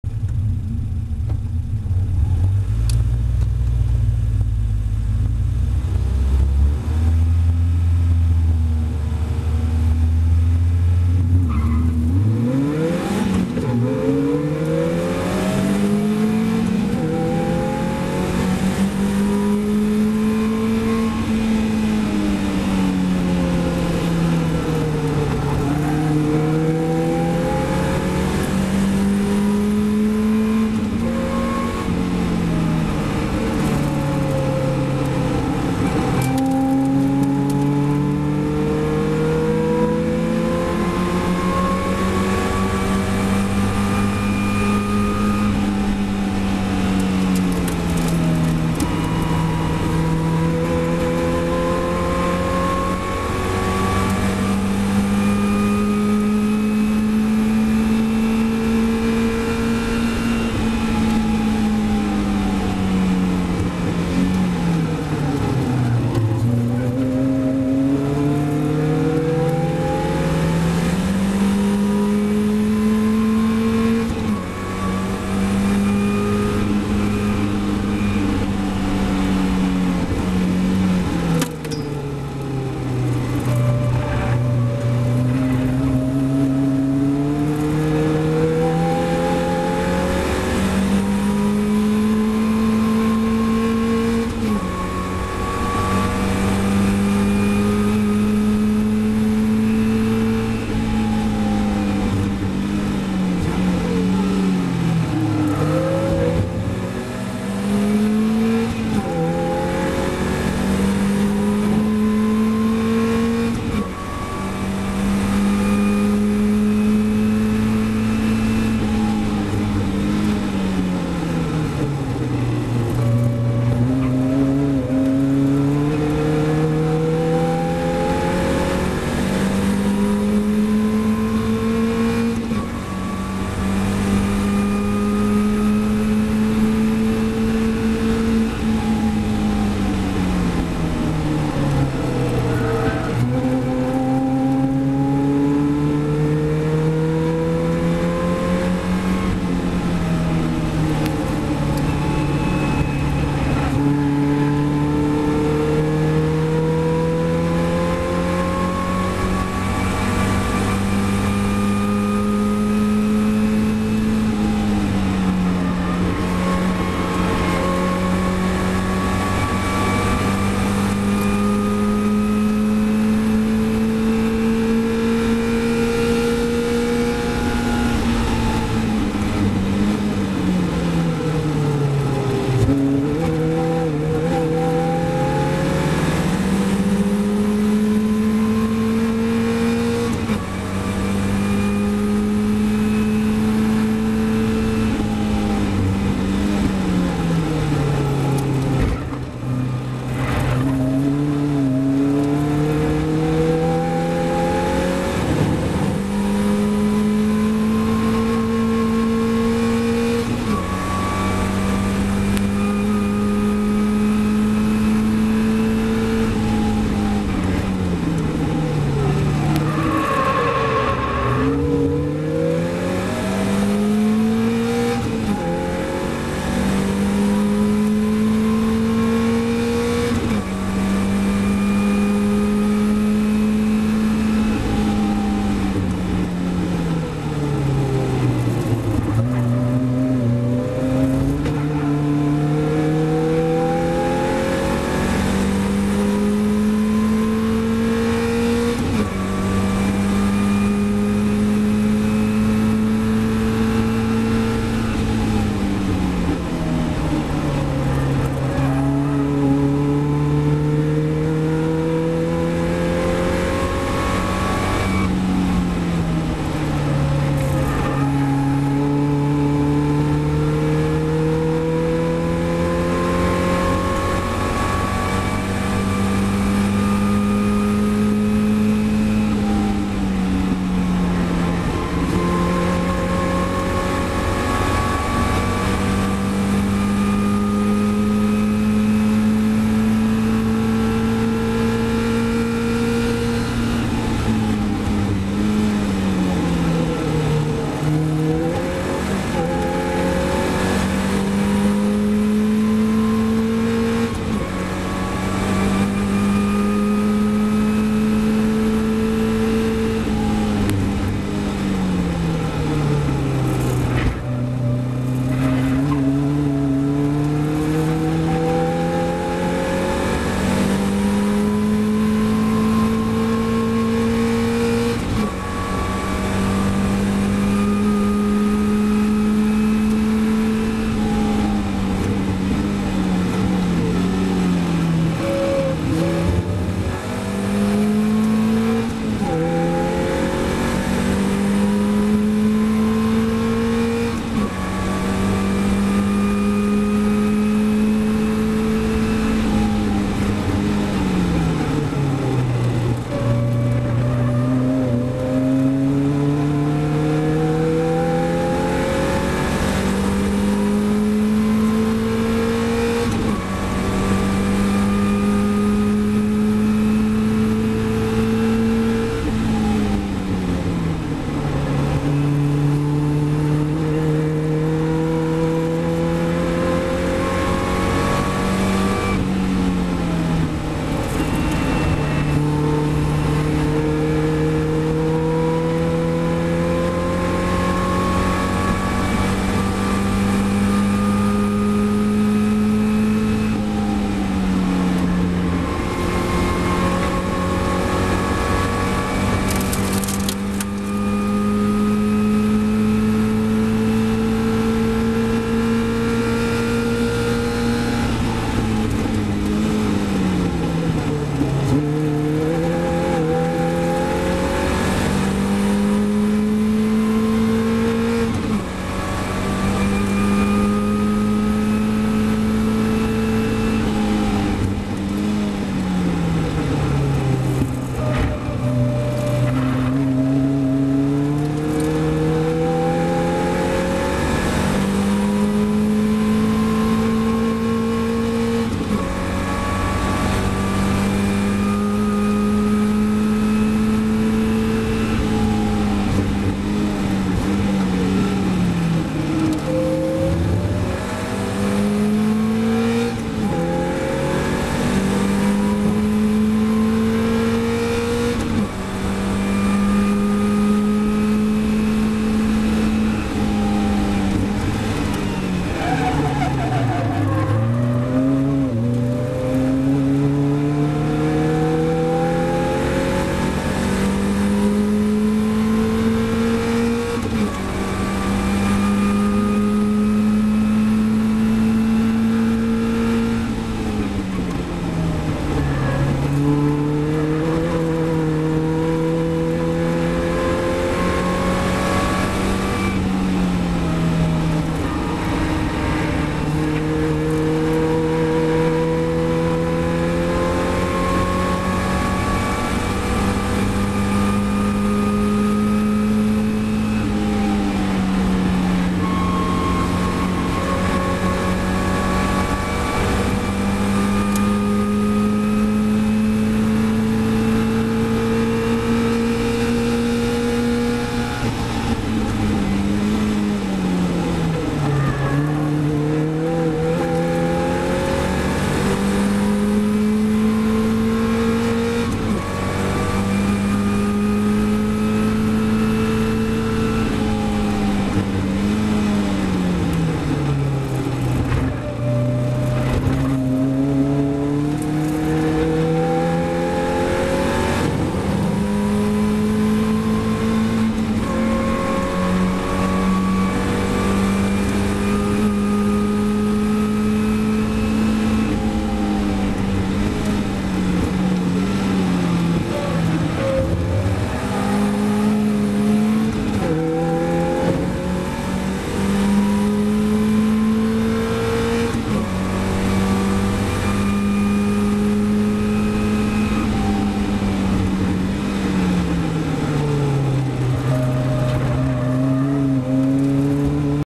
Zolder Race 1 pt1 Caterham Supersports 2010
The highs and (mainly) lows of Race1! Good 1st half..appaling 2nd half where I think I became possessed.